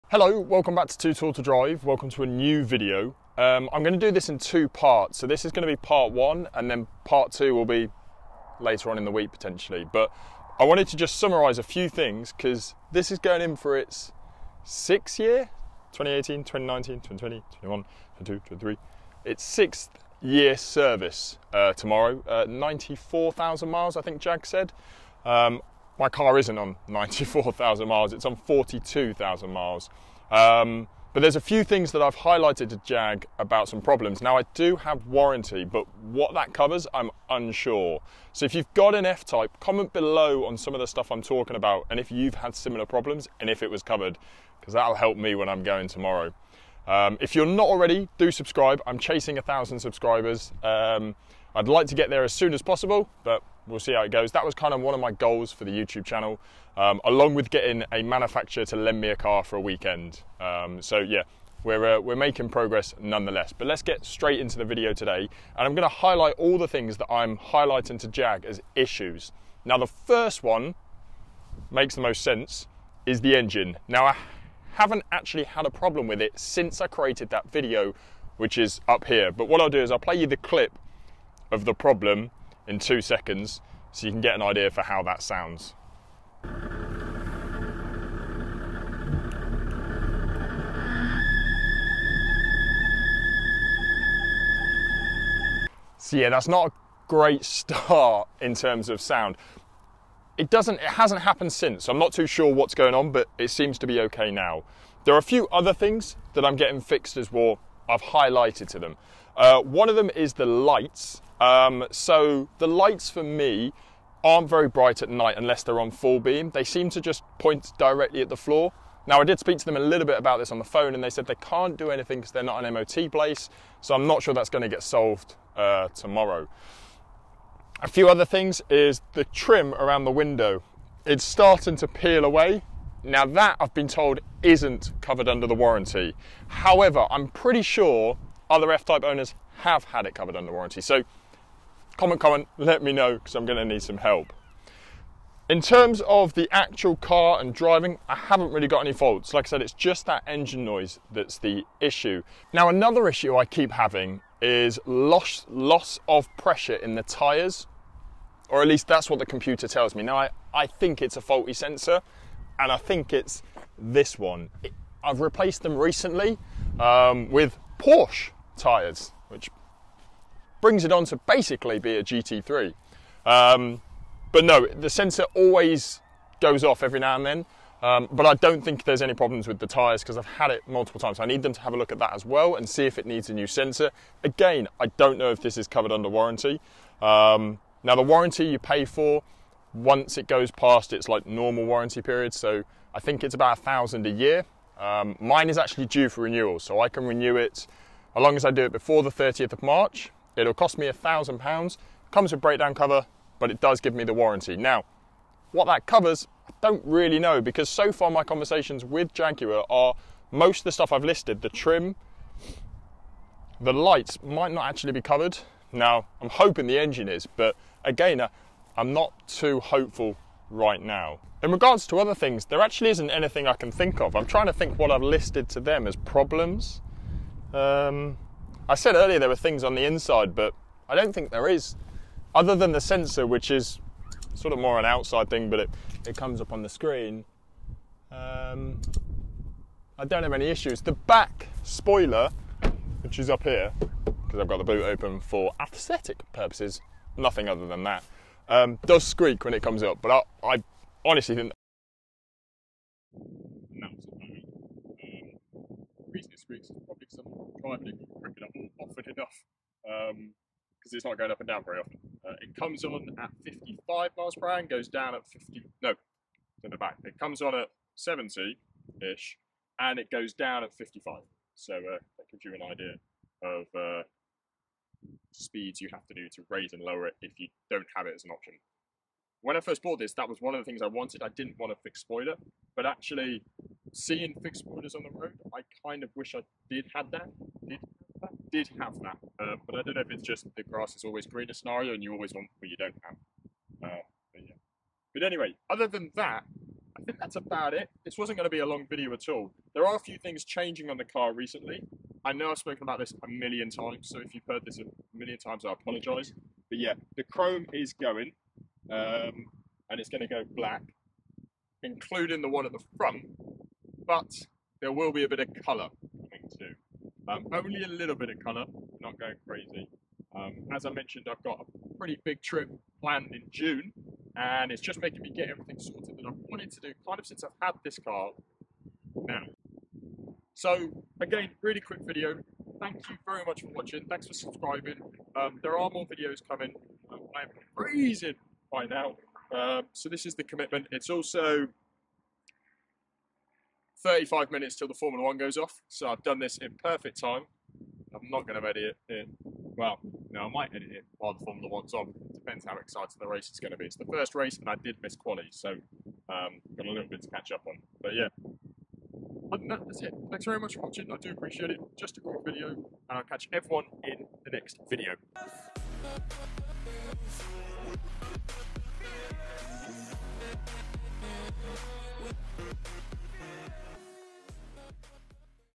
My Jaguar F-Type is due a service... [Part 1]
My Jaguar F-Type is going for a service and I list off some of the issues I have mentioned to Jaguar when it goes to them.

Apologies for the Audio at the end, not sure what happened. However, if when I hit 1000 subscribers I will be buying a camera with a mic input, so help me get there!